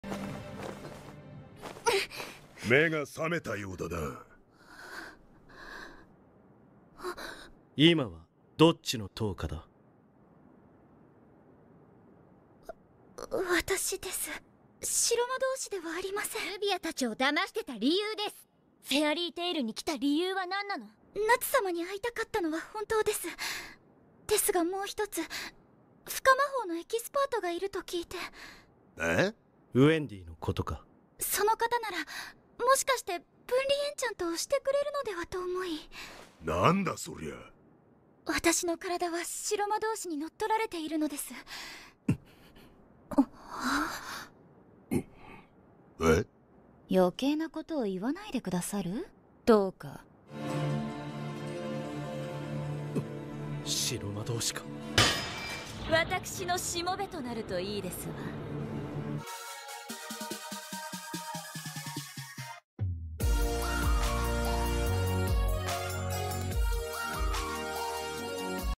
Touka's body under the White Wizard's spell
Discover what happens to Touka's body under the White Wizard's spell in this trending anime edit. Dive into the world of manga and anime with this viral video!

Description tags: